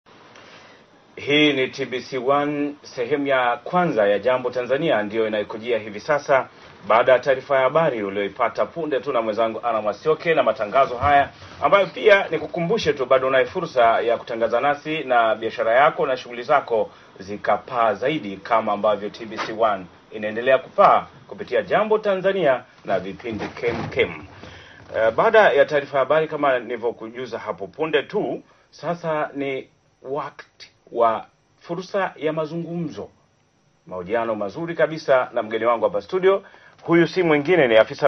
Siku Ya Kisomo Duniani
Fuatilia hapa ufahamu zaidi kuhusiana na maadhimisho ya siku ya kusoma duniani kutoka kwa Afisa mipango sekta ya elimu  Bi Faidhi Shayo